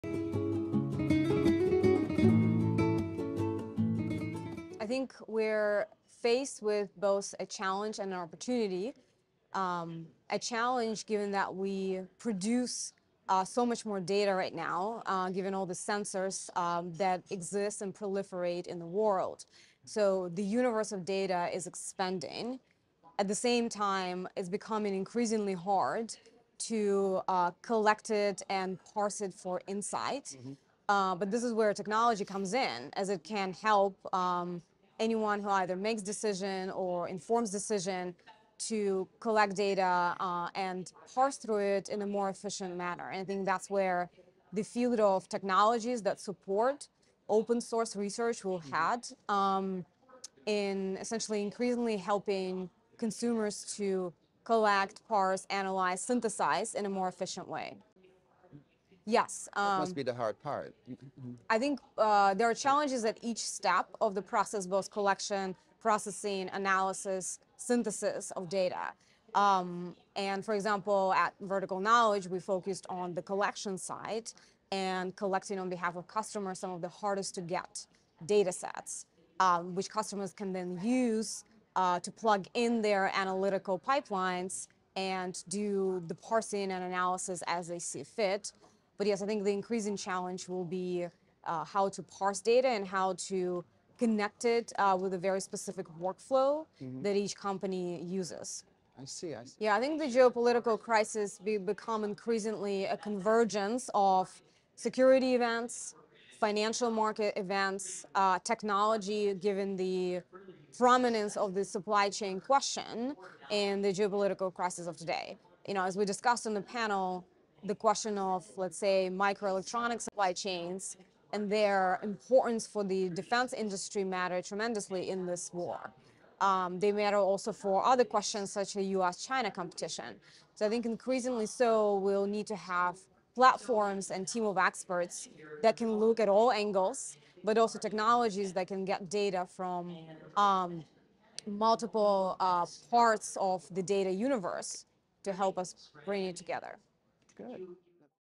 INTERVIEW 🎙️ Nataliya Bugayova, Vertical Knowledge discusses how Tech helps Collect/Synthesize Data
Interview featuring:
Nataliya Bugayova, Director of Strategic Insights
Michael Kappeyne, The Beryl Consulting Group